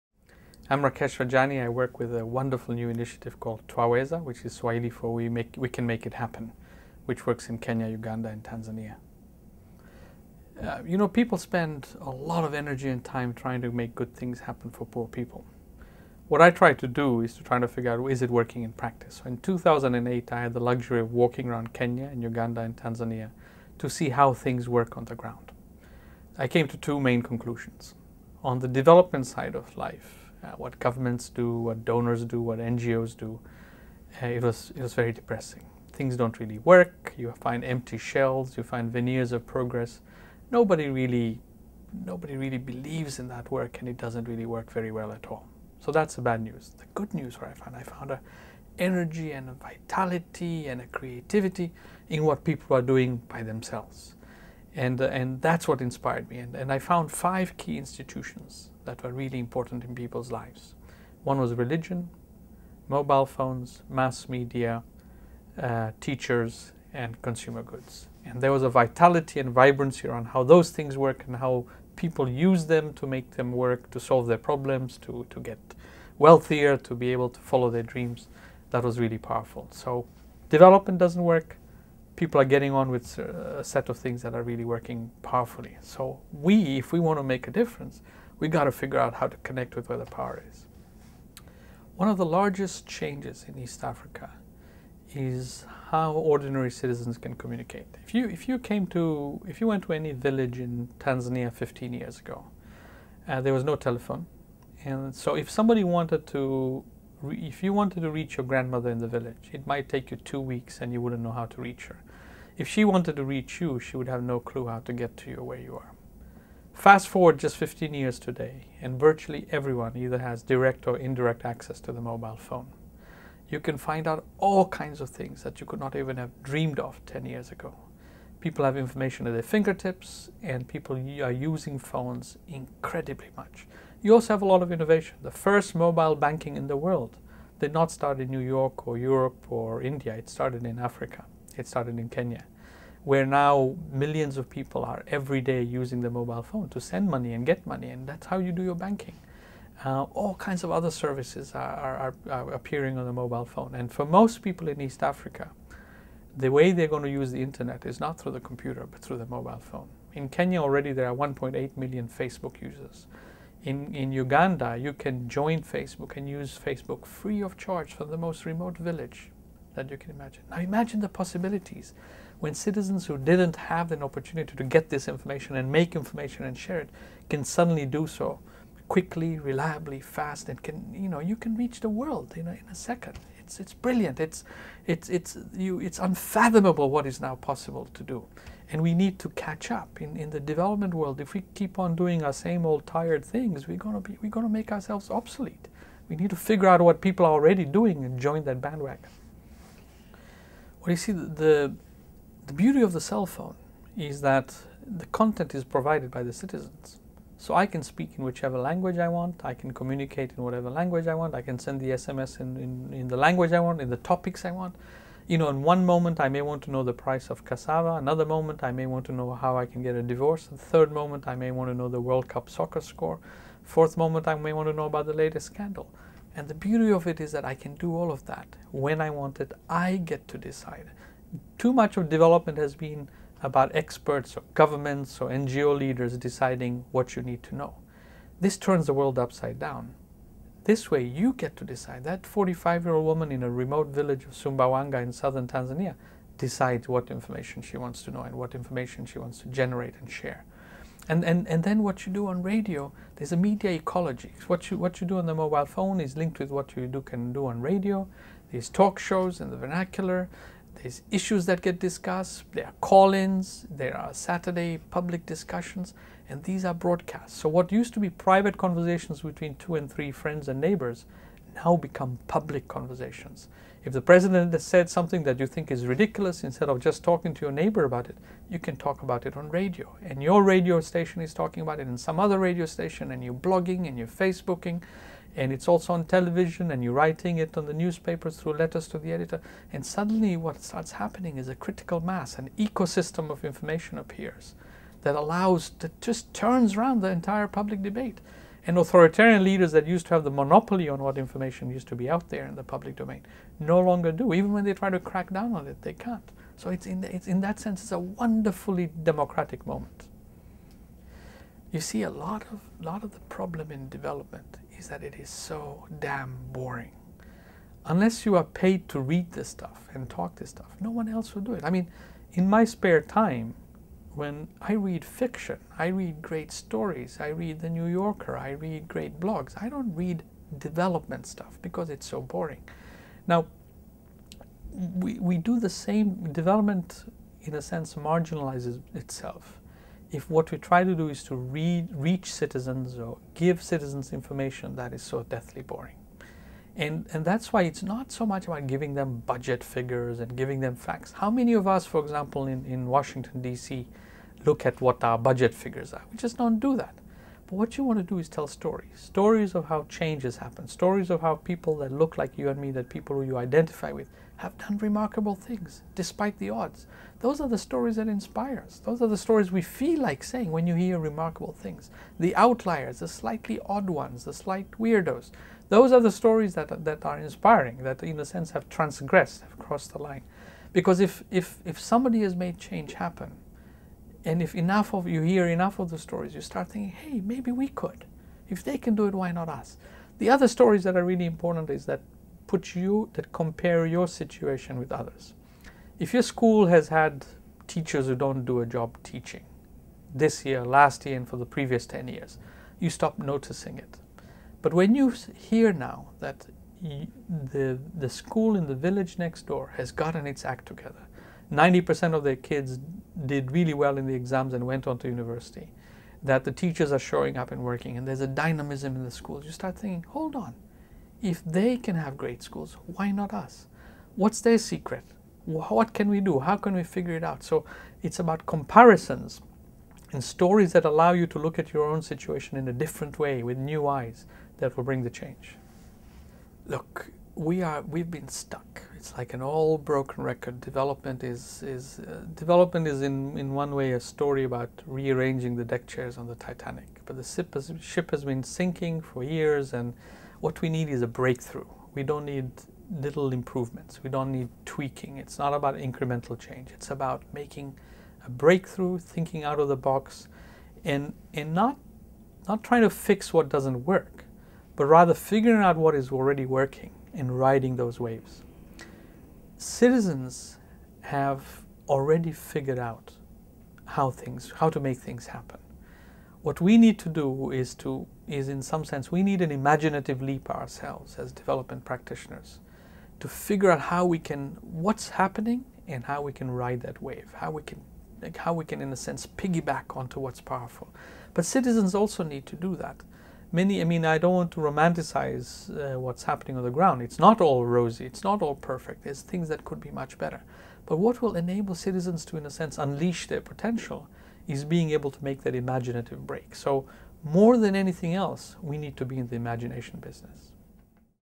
Rakesh Rajani talks about the power of innovation
Rakesh Rajani talks with Edith Wilson issue editor of Development Outreach: The Power of Innovation about the publication.